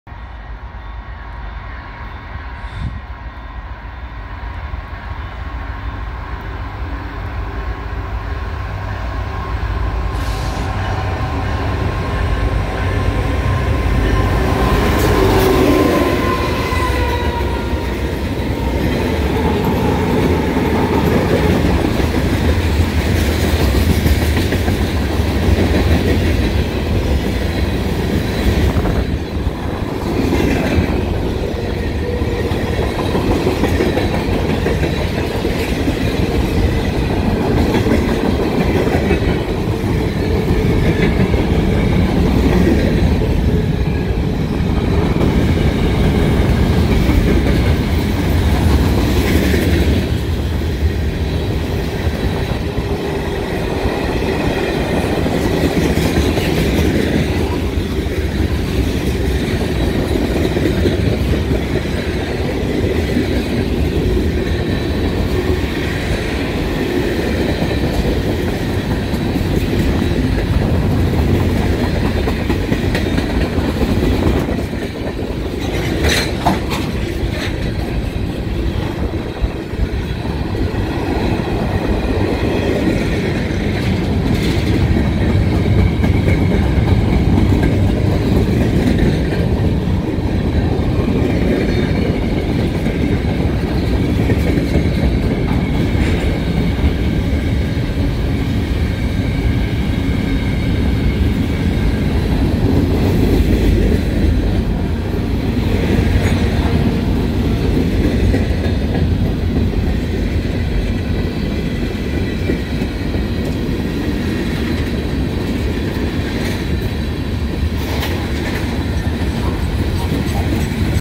CP 287 with BNSF ACe & 2 CP SD70ACus
JLT trains and I barely got here in time from running his new Genesis UP 4141 on his layout when I heard the ACus were close!